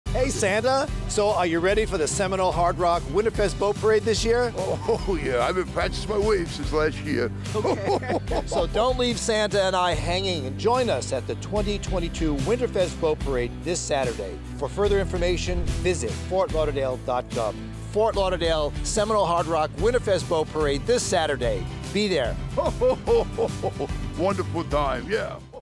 The Best Show on H2O is back!
The Best Show on H2O is coming to a shore near you this weekend! ⛵ 🚤 ⛴️ 🛥️ The Winterfest Boat Parade will be floating through FortLauderdale Saturday 12/10 starting at 6:30 pm.

For info on viewing area tickets, visit: 🔗 ftlcity.info/3FahVh8
For info on bridge & waterway closings & more, visit: 🔗fortlauderdale.gov/winterfest